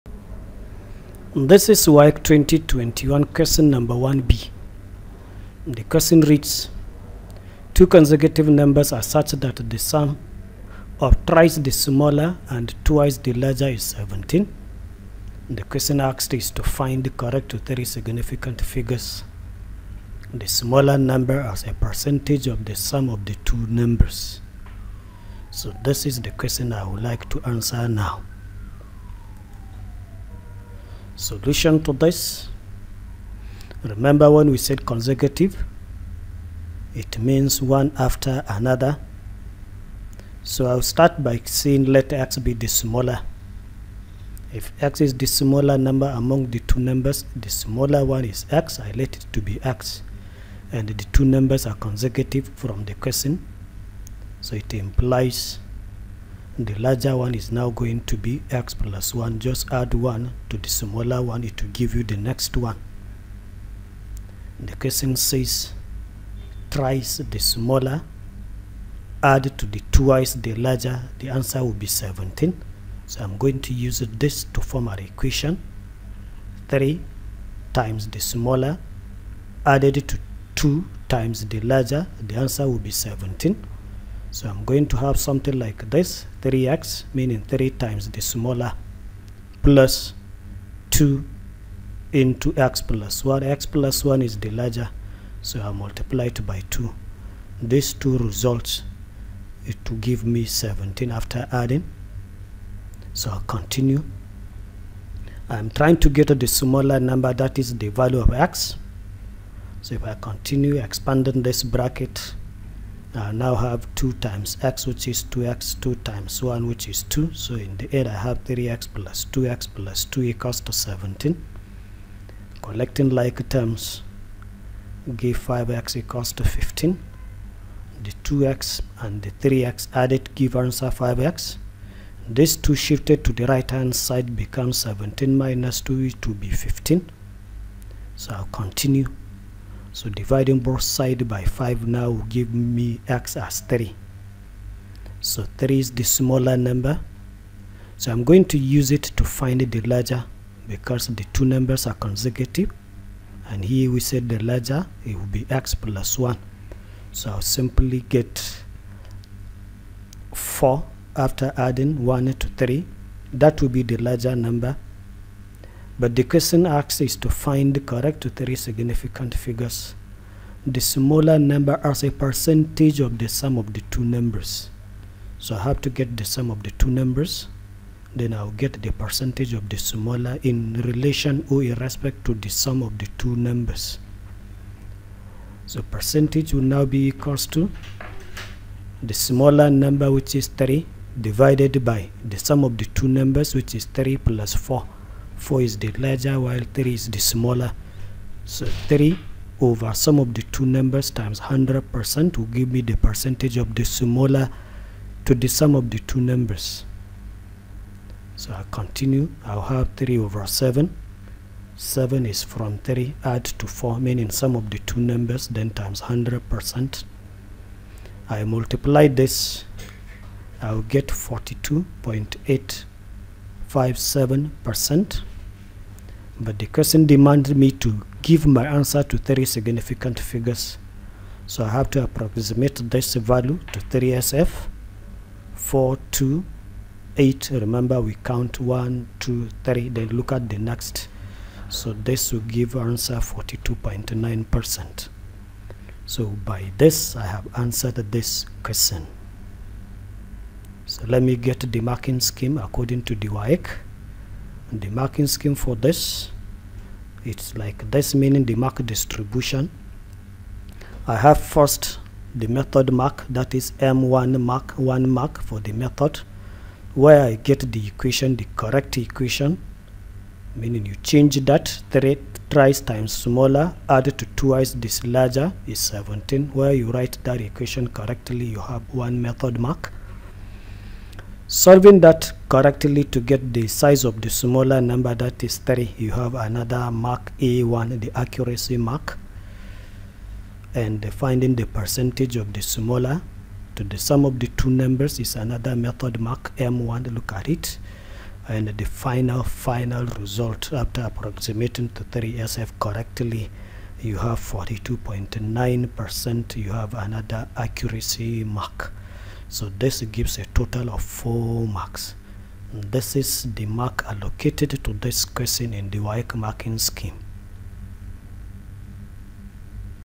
WAEC 2021 GENERAL MATHEMATICS 2 | WASSCE | MYEDUMARKET
In this video we solve clearly question 1 b of WAEC 2021 General Mathematics paper 2 and explain in detailed the mark distribution according the marking scheme.